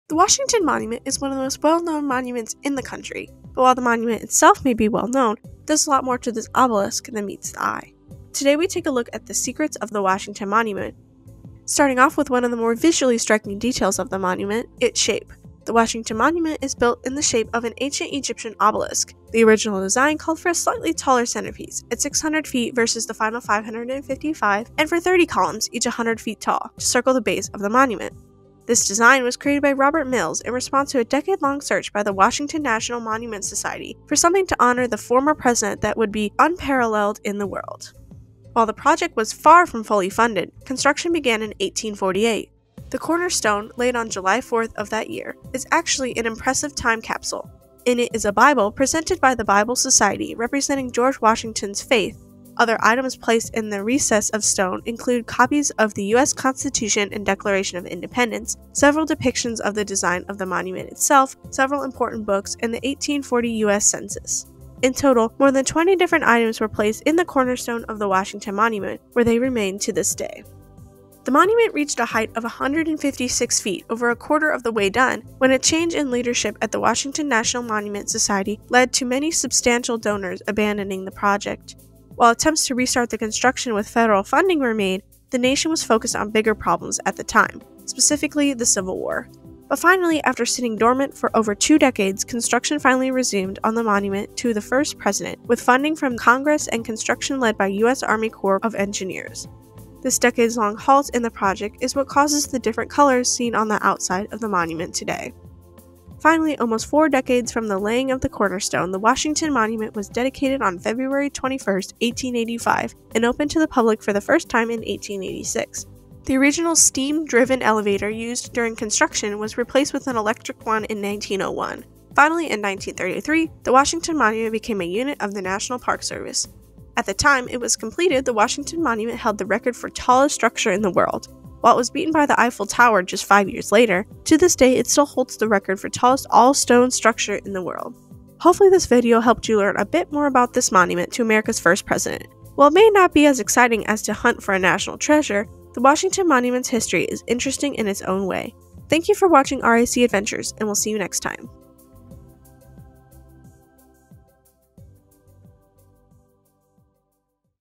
The Secrets of the Washington Monument
The Washington Monument is one of the most well known monuments in the country. But while the monument itself may be well known, there’s a lot more to this obelisk than meets the eye. Today, we take a look at the secrets of the Washington Monument.